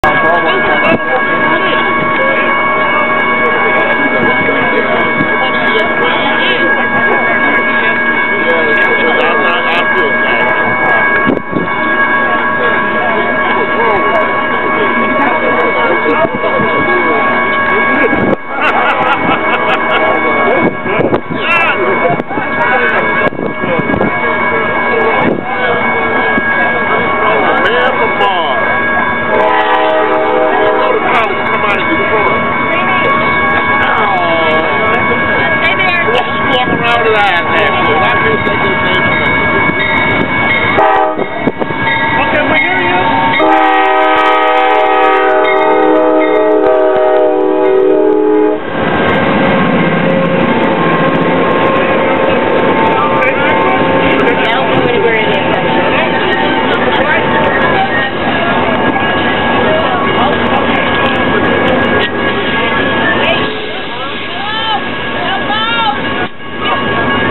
Amtrak 717 arriving at Colonial Allensworth State Park
Whistle stop at CNL on October 11, 2008 - about 45 minutes late